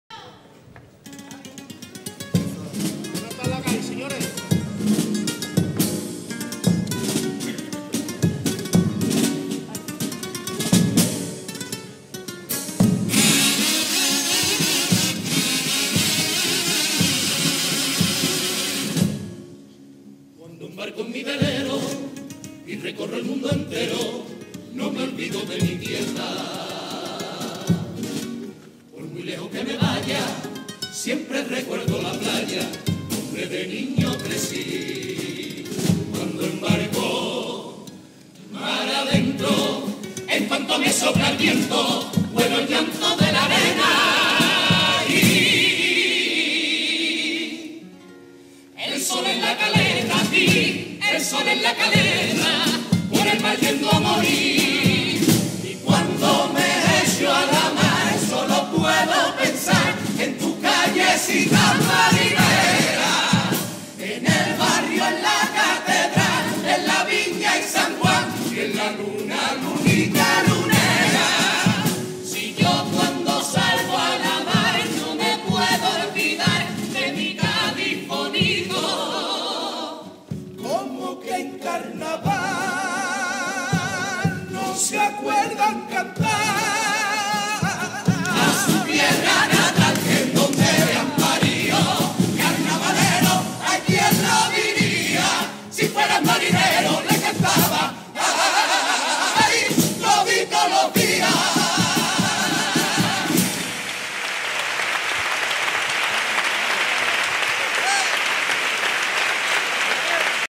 Comparsa “Lobos”: Segundo Pasodoble | SEMIFINALES del Carnaval de Cádiz 2015
Segundo pasodoble, dedicado a Cádiz, que la comparsa de Juan Fernández ha presentado en la Segunda sesión de Semifinales del Carnaval de Cádiz 2015.